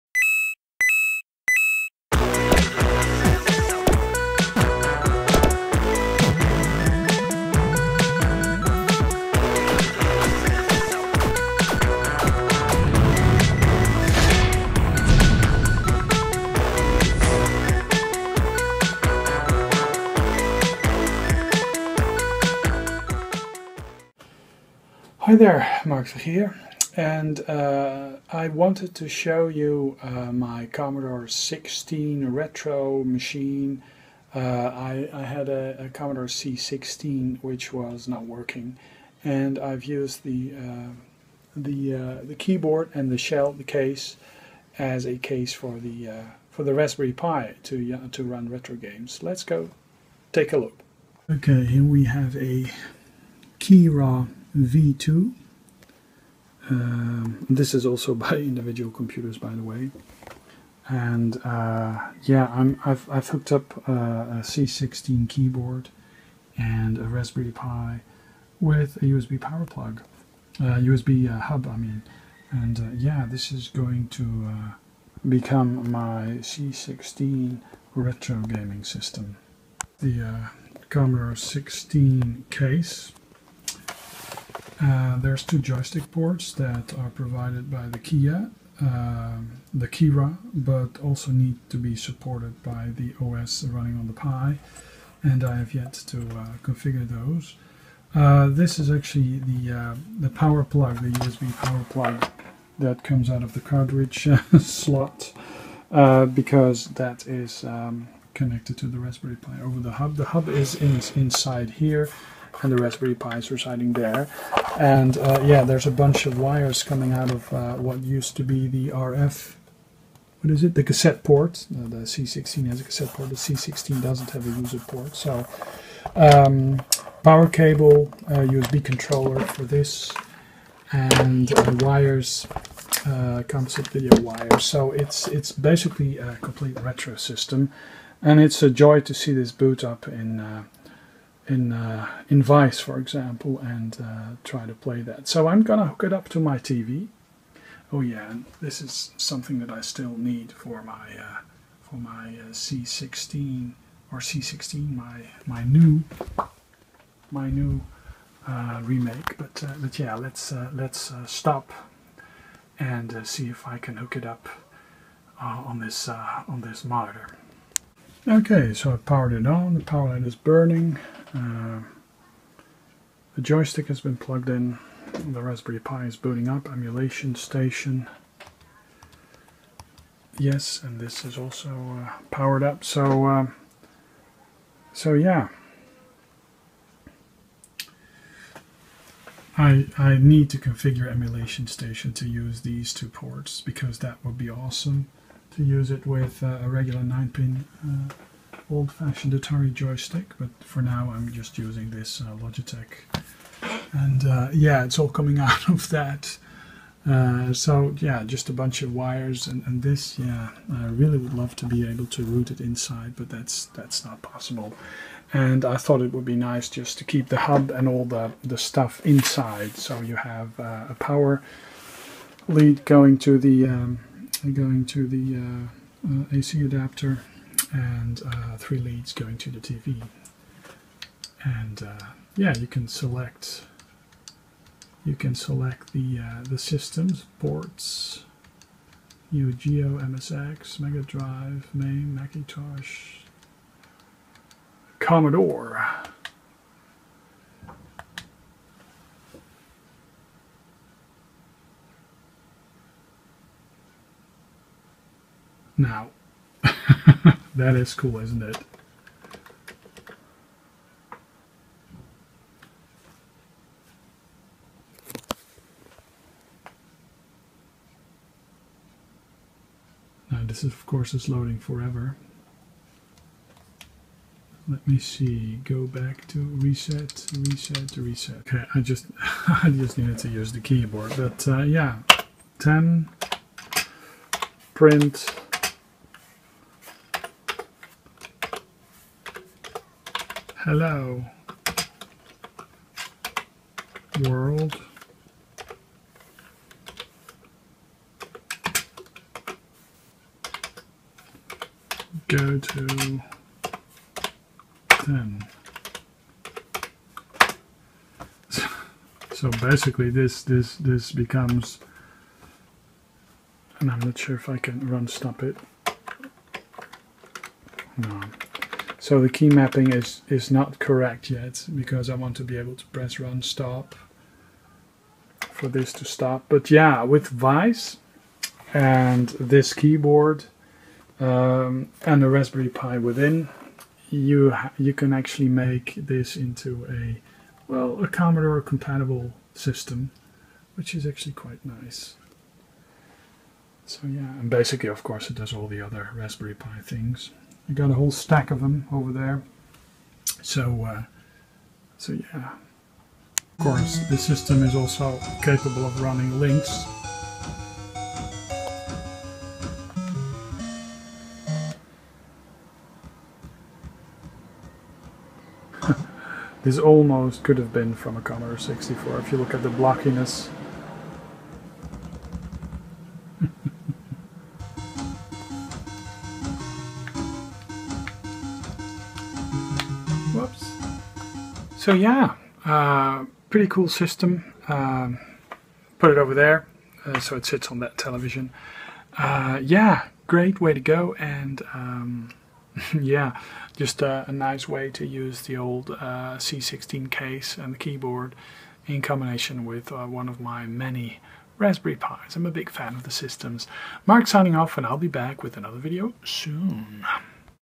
RetroPie C16 Case Project for the Raspberry Pi
I decided to put my dead Commodore C16's case and keyboard to good use turning them into a working retro computer. Check it out.

Here's where you can find the Keyrah V2 that I used in this project: